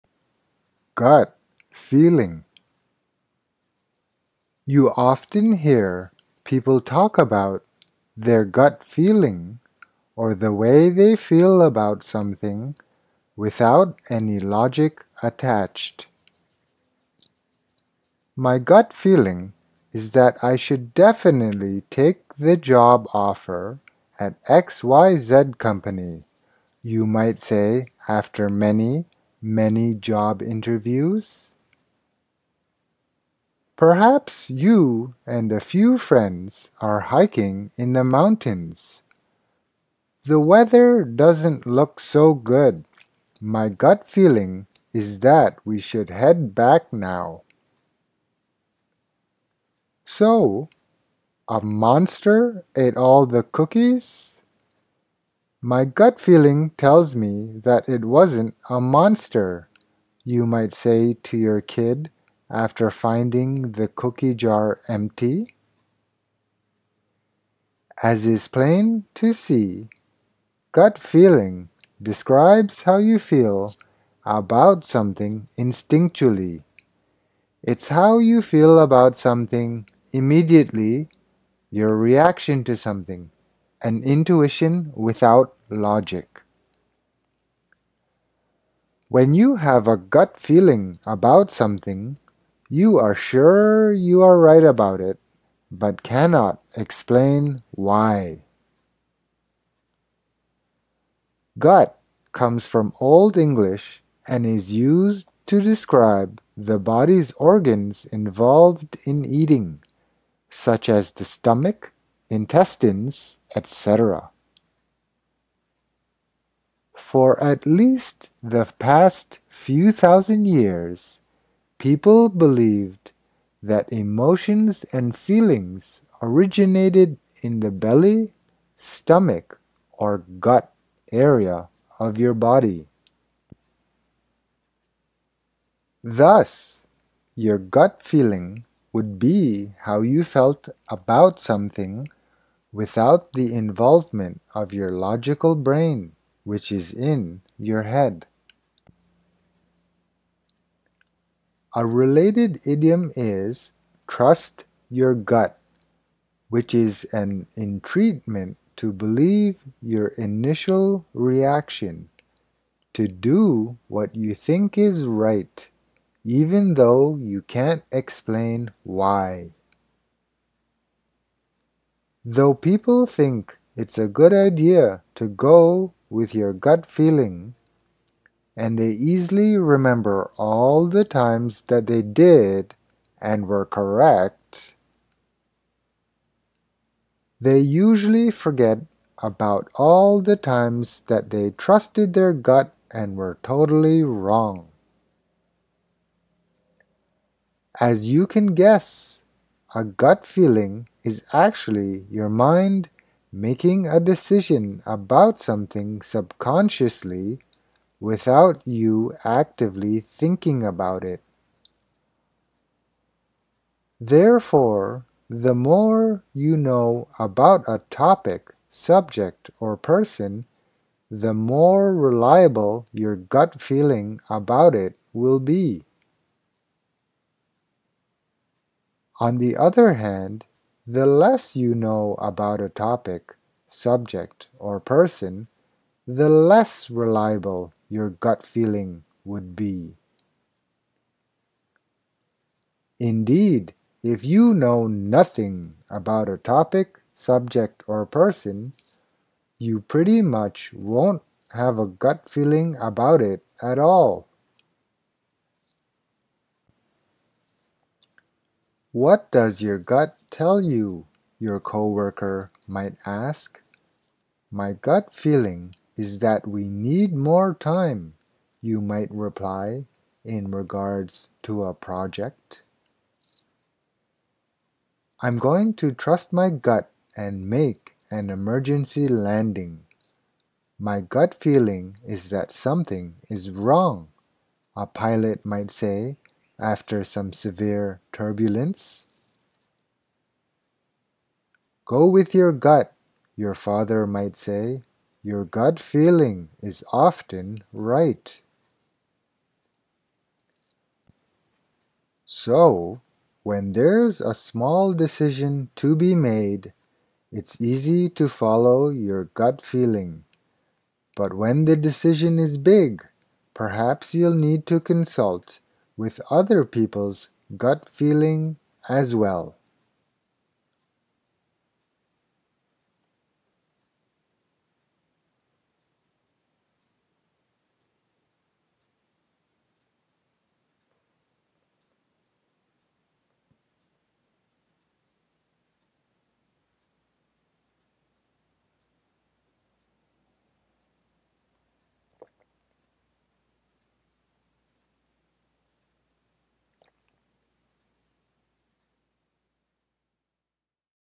"Gut Feeling" Idiom Meaning, Origin & History | Superduper English Idioms
0:00 - How to Say, “Gut feeling…”
0:17 - Examples of “Gut feeling…”
1:06 - Meaning of “Gut feeling…”
1:40 - Origin of “Gut feeling…”
4:17 - More Examples of “Gut feeling…”
5:00 - Conclusion

“Gut feeling…”

You often hear people talk about their “gut feeling”, or the way they feel about something without any logic attached.

“My gut feeling is that I should definitely take the job offer at XYZ Company,” you might say after many, many job interviews.

Perhaps you and a few friends are hiking in the mountains, “The weather doesn’t look so good, my gut feeling is that we should head back now.”

“So, a monster ate all the cookies? My gut feeling tells me that it wasn’t a monster,” you might say to your kid after finding the cookie jar empty.

As is plain to see, “gut feeling” describes how you feel about something instinctually. It’s how you feel about something immediately, your reaction to something, an intuition without logic.

When you have a “gut feeling” about something, you are sure you are right about it, but cannot explain why.

“Gut” comes from Old English and is used to describe the body’s organs involved in eating; such as the stomach, intestines, etc.

For at least the past few thousand years, people believed that emotions and feelings originated in the belly, stomach or “gut” area of your body.

Thus, your “gut feeling” would be how you felt about something without the involvement of your logical brain, which is in your head.

A related idiom is, “trust your gut,” which is an entreatment to believe your initial reaction, to do what you think is right - even though you can’t explain why.

Though people think it’s a good idea to go with your “gut feeling” - and they easily remember all the times that they did and were correct…

… they usually forget about all the times that they “trusted their gut” and were totally wrong.

As you can guess, a “gut feeling” is actually your mind making a decision about something subconsciously, without you actively thinking about it.

Therefore, the more you know about a topic, subject or person, the more reliable your “gut feeling” about it will be.

On the other hand, the less you know about a topic, subject or person, the less reliable your “gut feeling” would be.

Indeed, if you know nothing about a topic, subject or person, you pretty much won’t have a “gut feeling” about it at all.

“What does your gut tell you?” your coworker might ask. “My gut feeling is that we need more time,” you might reply in regards to a project.

“I’m going to trust my gut and make an emergency landing. My gut feeling is that something is wrong,” a pilot might say after some severe turbulence.

“Go with your gut,” your father might say, “your gut feeling is often right!”

So, when there’s a small decision to be made, it’s easy to follow your “gut feeling”, but when the decision is big, perhaps you’ll need to consult with other people’s “gut feeling”, as well!